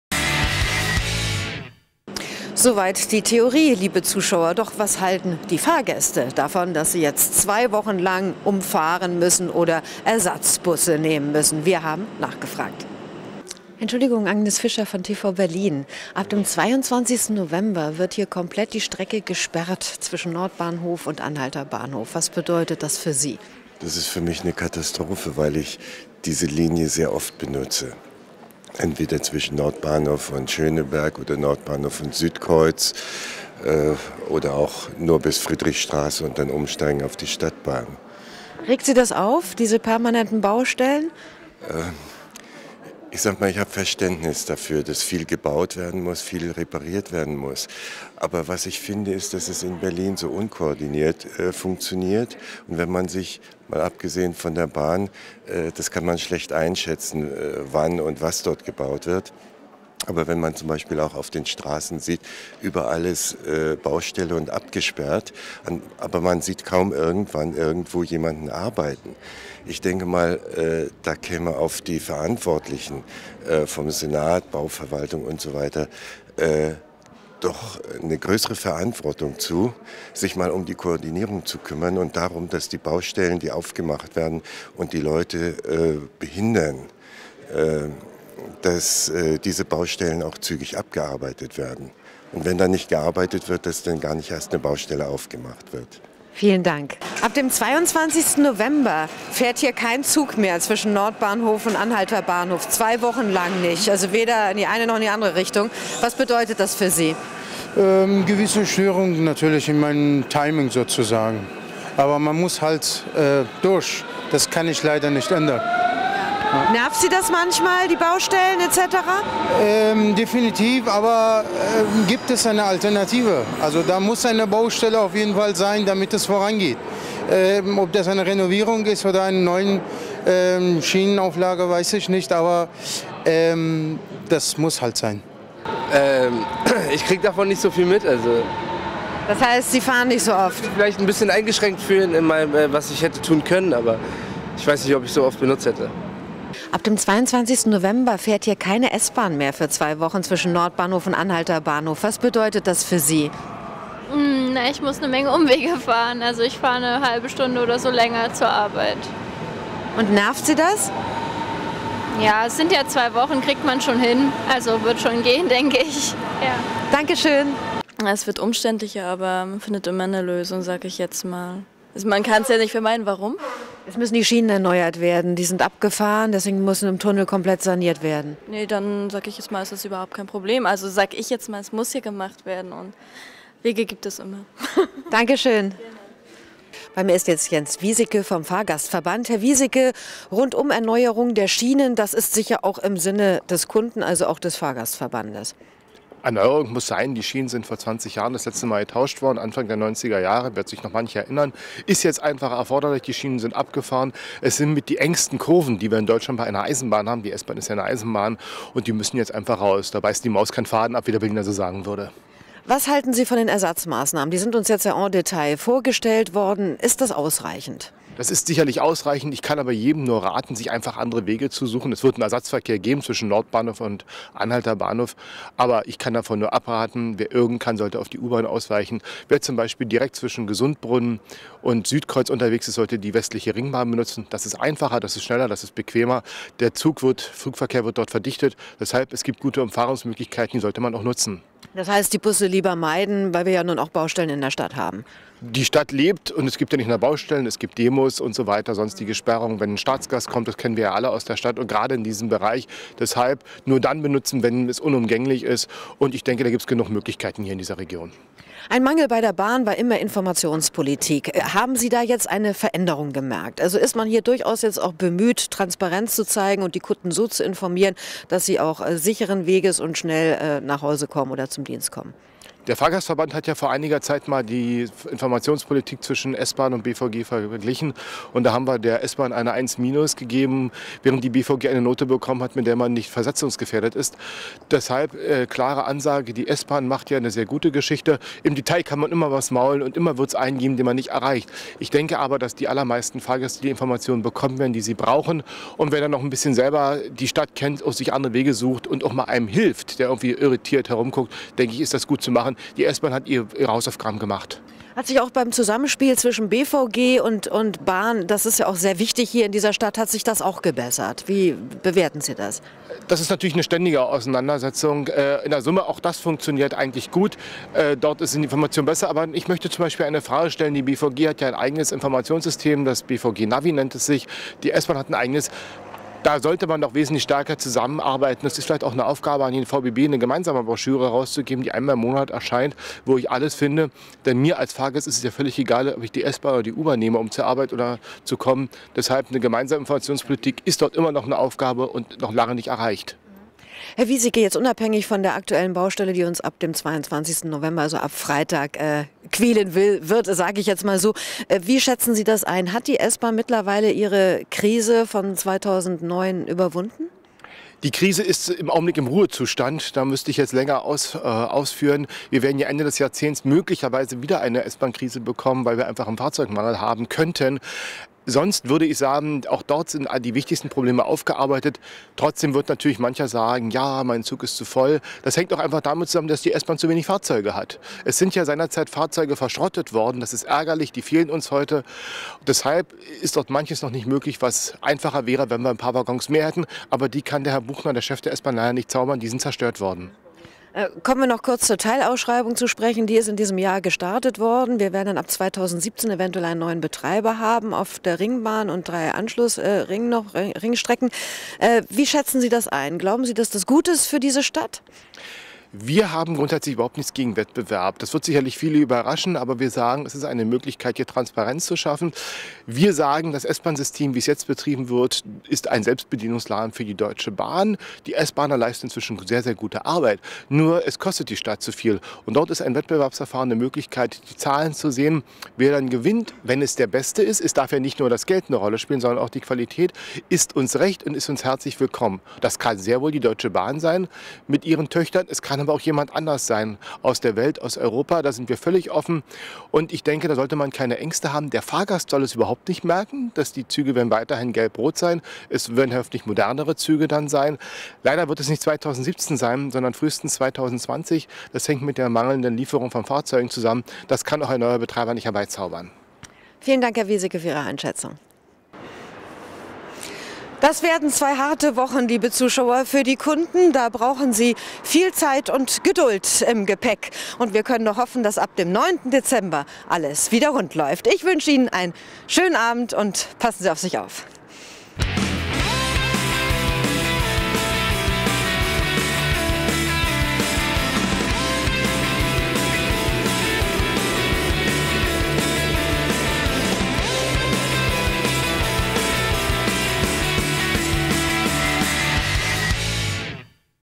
S-Bahn: Sperrung der Nord-Süd-Verbindung (Kiez konkret) - Teil 3
Gerade freuen sich die Nord-Süd-Pendler über eine ununterbrochene U6-Fahrt, da müssen sie sich wieder auf eine neue Entwicklung einstellen: Die S-Bahnlinien 1,2 und 25 werden ab Freitag zwischen Nordbahnhof und Anhalter Bahnhof unterbrochen. Zwei Wochen lang müssen die Fahrgäste der S-Bahn deswegen auf Ersatzbusse ausweichen. Lange Wartezeiten und volle Busse sind da vorprogrammiert. Das meint sogar der Fahrgastverband, der empfiehlt, die Ersatzbusse zu vermeiden.